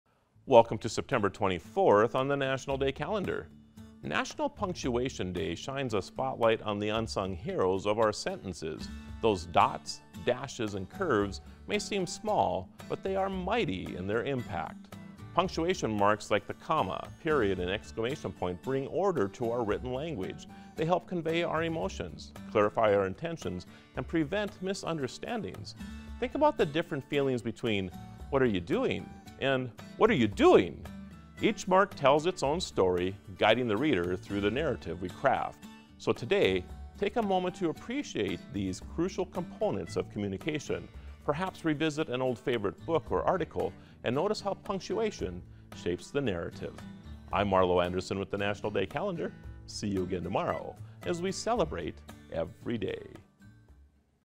NATIONAL PUNCTUATION DAY | September 24th - National Day Calendar
September 24th is National Punctuation Day!
Marlo Anderson shares his fun insight on this sentence-ending national day.

READY TO JOIN IN ON THE CELEBRATION?

Don't forget to celebrate every day!